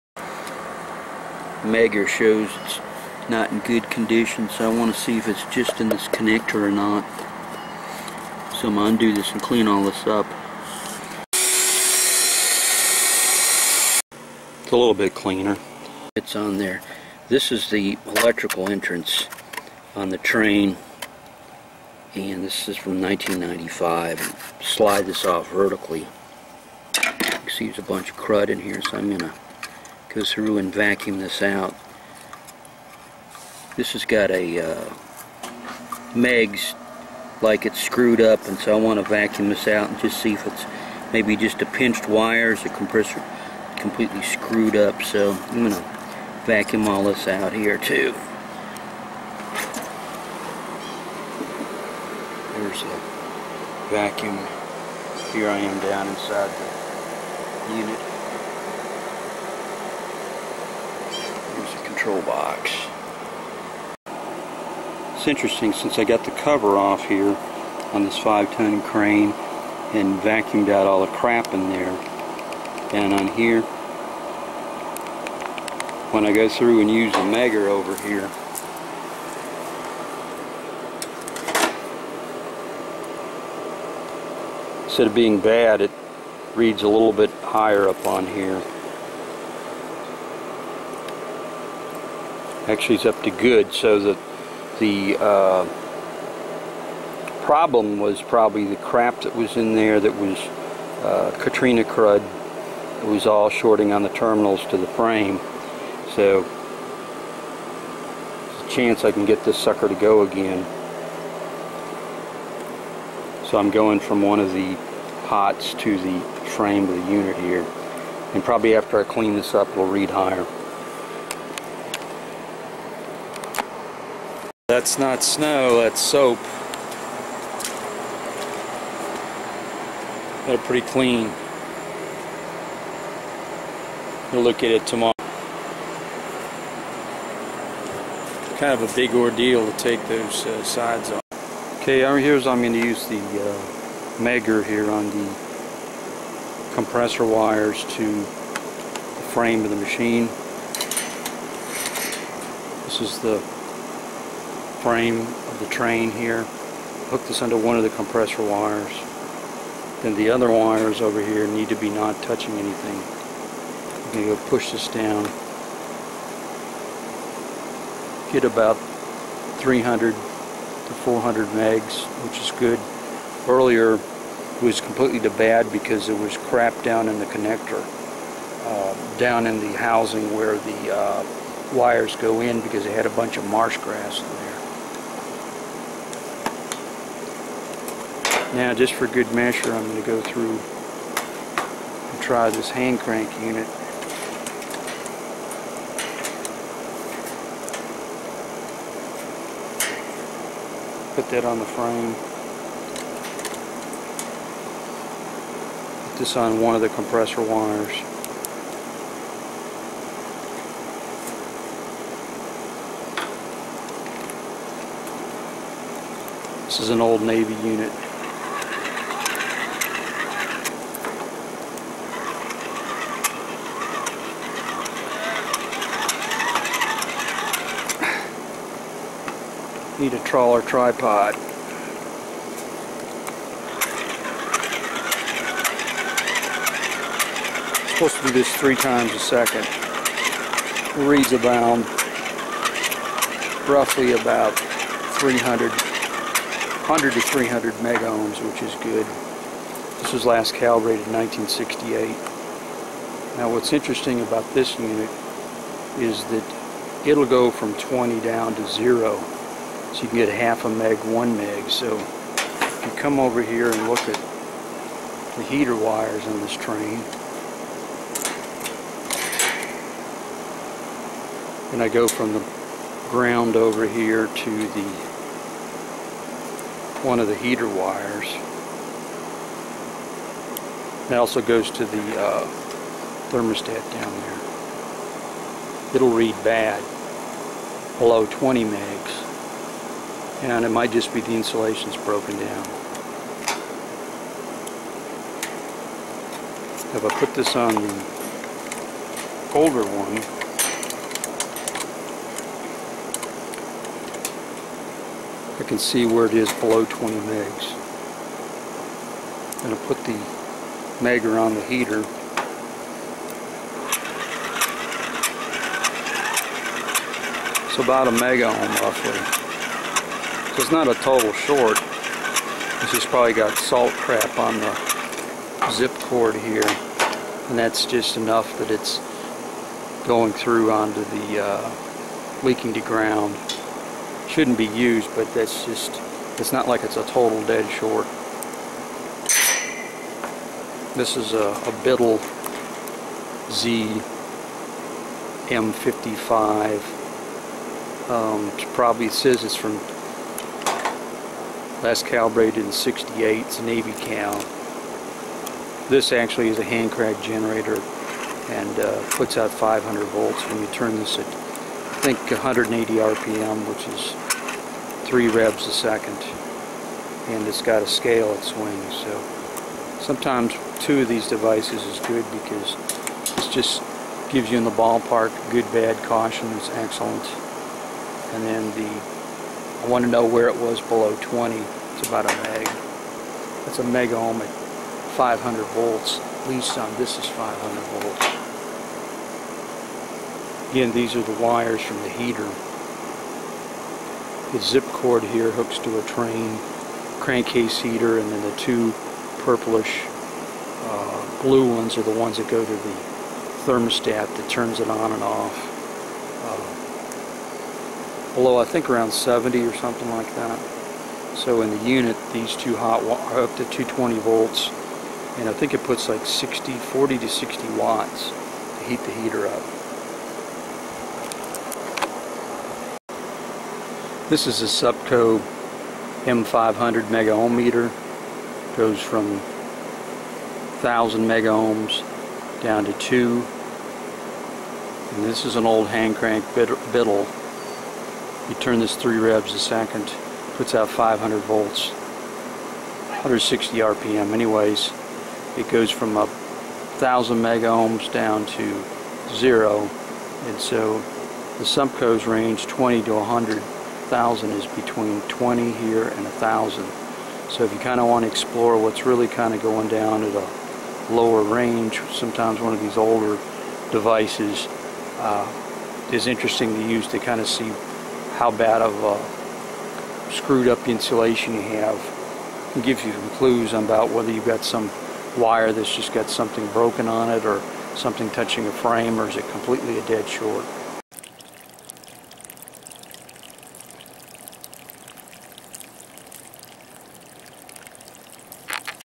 HVAC Bad Megger readings on Trane were due to crud
Compressor had bad Megger  Supco MM500 readings until wire entrance area vacuumed and washed and dried. Katrina salt water marsh grass was still under this cover. Via the Supco MM500 original reading BAD less than 20 meg. After vacuuming 100 megs. After washing  and drying 300 to 400 megs.